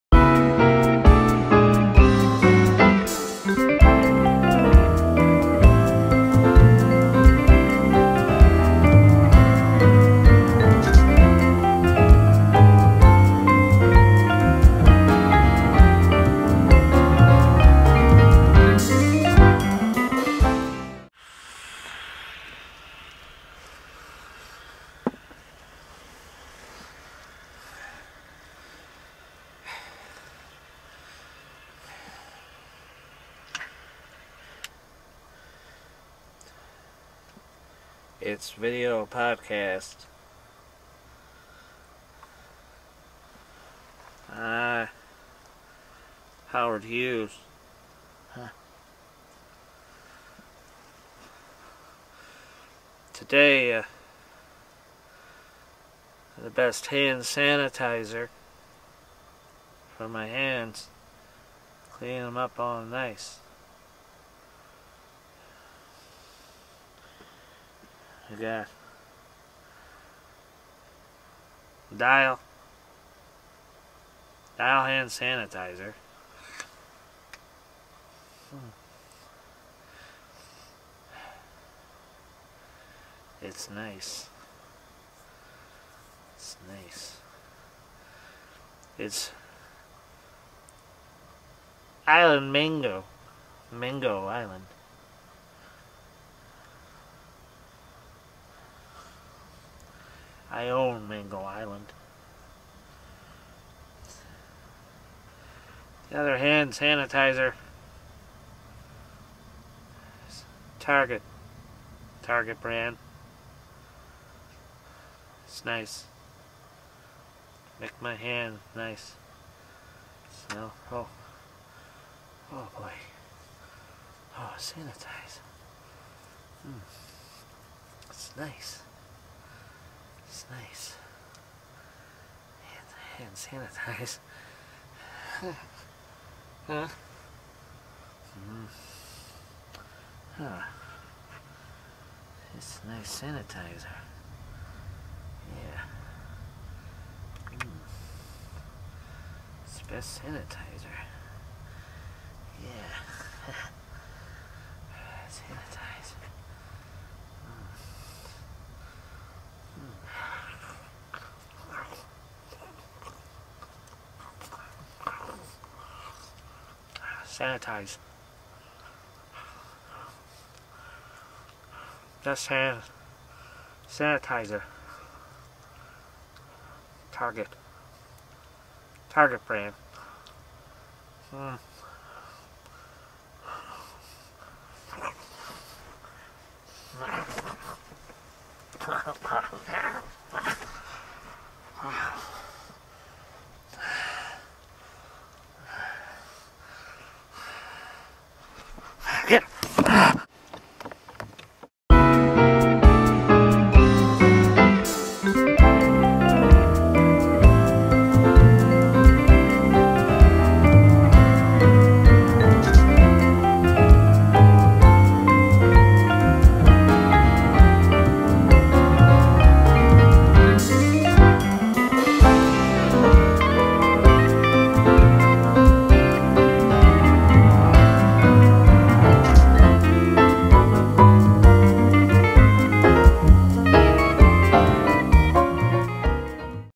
The Howard Hughes Video Podcast # 1
Thought long dead, the very alive and reclusive, Billionaire Howard Hughes cheerfully reviews the bast hand sanitizer on the market today.